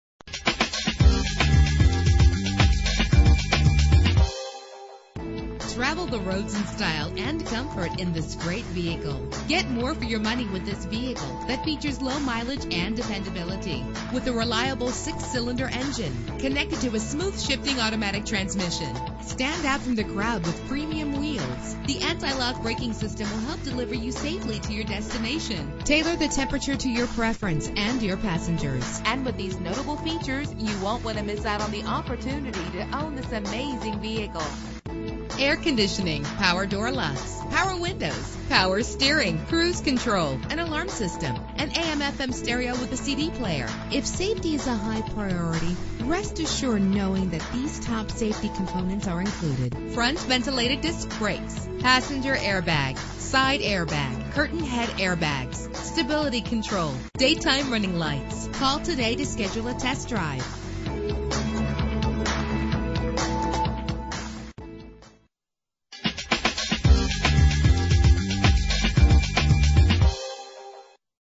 2010 Lexus RX 450h - Annapolis MD
Year: 2010
Make: Lexus
Model: RX 450h
Trim: 450
Engine: 3.5 liter 6 cylinder 24 valve
Transmission: Automatic CVT
Color: White
Mileage: 12713
Address:
121 Ferguson Rd.